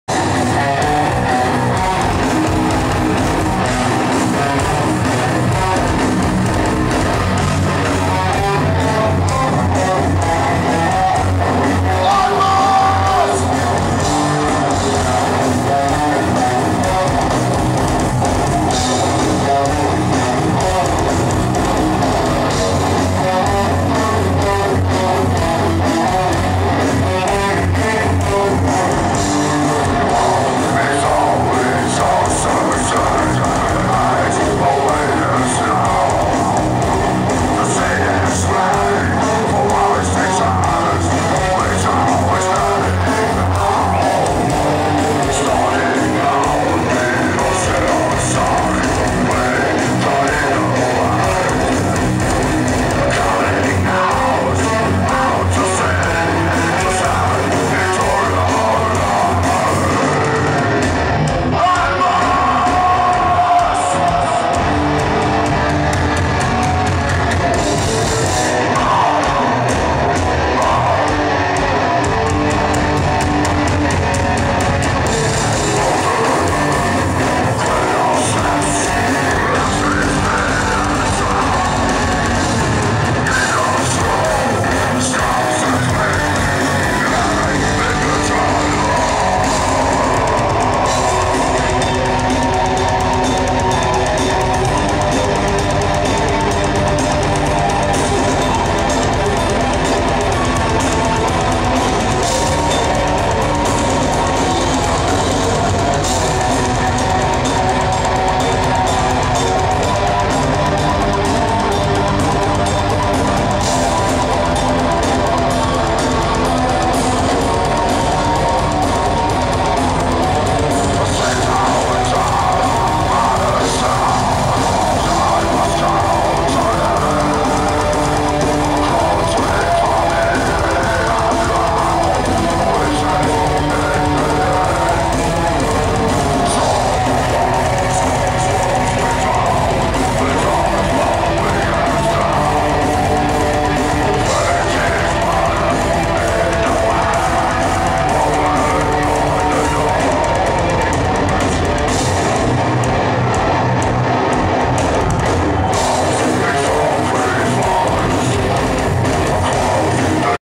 DESTINITY live at Annemasse (FR) (Chateau rouge)
Amon amarth cover song - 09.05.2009